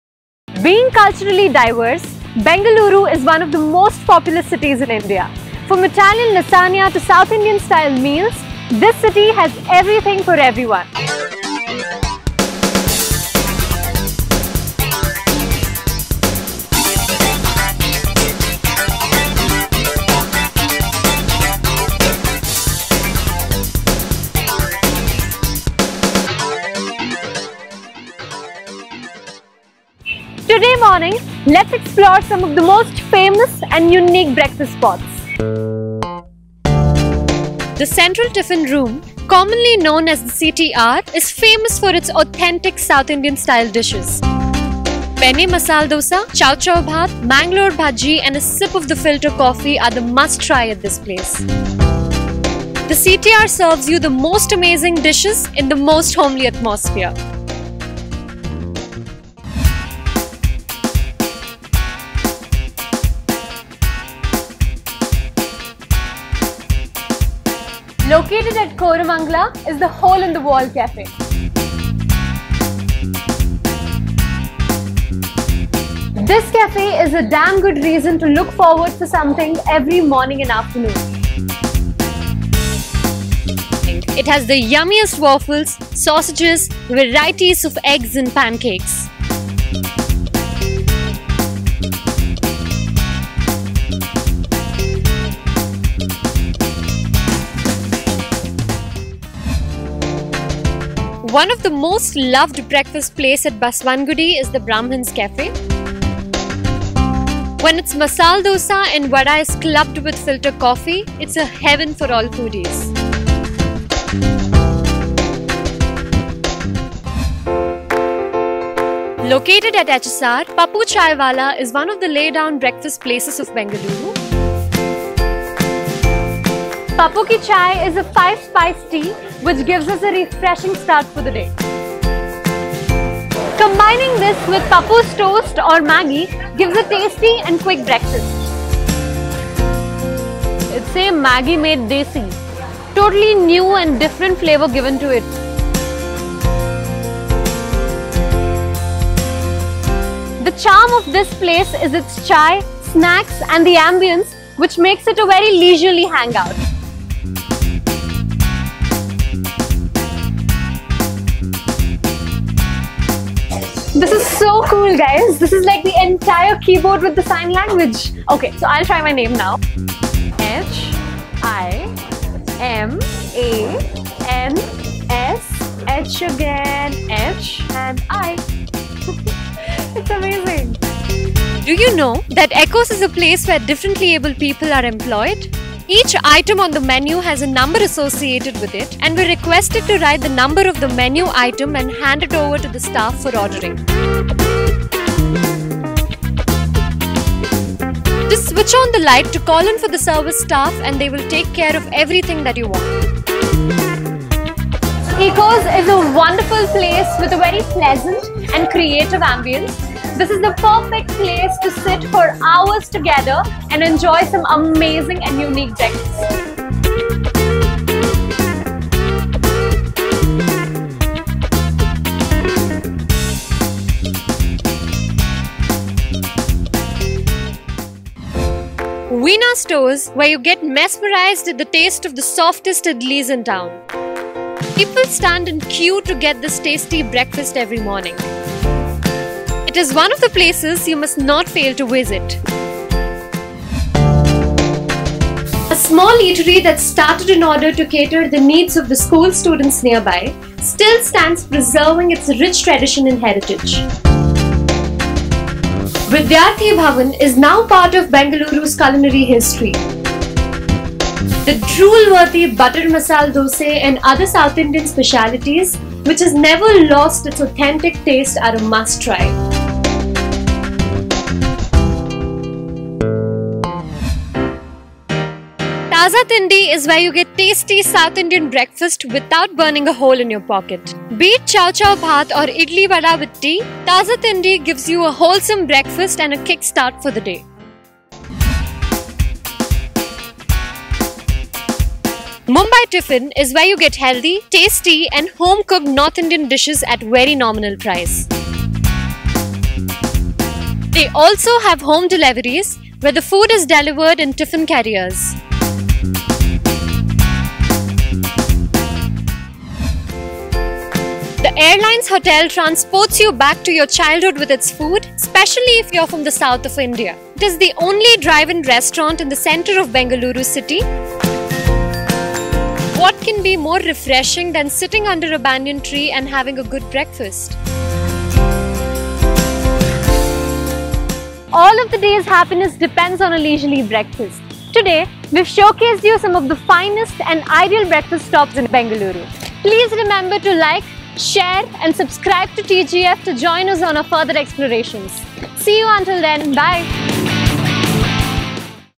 Iconic Breakfast Places in Bengaluru | Best Breakfast places in Bangalore | Must Have Breakfast
All happiness depends on a leisurely BREAKFAST!
Breakfast being the most important meal of the day, the city has a number of outlets that offer the best and breakfast, ranging from South-Indian delicacies to proper English meals - thus making Bengaluru a favourite place for everyone. If food is what you crave - begin with having breakfasts at these places in namma Bengaluru !

Join TGF, as we take you to some of the  unique food-gasmic breakfast stops in Bengaluru which offers the tastiest breakfast and set you up for a fun filled day. Do try these amazing breakfast joints while we find more exciting places for you to explore!

Place Covered:-
1. Central Tiffin Room, CTR(Malleshwaram) - Must try Benne masala dosa ,chow chow bhat, mangalore bajji and filter Coffee
2. The Hote in the Wall (4th block, Kormangala)- yummiest waffles, sausages, varieties of eggs, cakes and muffins.
3. Brahmin's Cafe (Basvangudi)- Must try their Idilis
4. Pappu Chaiwalla (HSR) - Must try their omelette kheema pav, Pappu ki Arrabiata maggi and vada pav. Pappu ki chai is a five spiced tea which gives us a refreshing start for the day.
5. Echoes (Kormangla)- a place where differently abled people are employed. Some of their must try's are Kitkat Milkshake, Red Velvet Milkshake, Kebab and Roti, Chinese Box
6. Veena Stores(Margossa Rd, Malleshwaram)- their softest idlis in are a must try.
7.Vidyarthi Bhavan(Gandhi Bazar, Basvangudi)-  butter masala dosae and the other south indian specialities are must try's.
8. Taaza Thindi(Jayanagar 4th block)-Their chowchow bhat, idly vada with tea is a must try.
9. Mumbai Tiffin(HSR) - get healthy, tasty and home cooked north indian dishes at very nominal price.
10. Airlines Hotel(Lavelle Road)-  transports you back to your childhood with its food, especially if you are from the south of India.

Explore your city and have a look at our video travel guides on TGF - The Guiding Factor.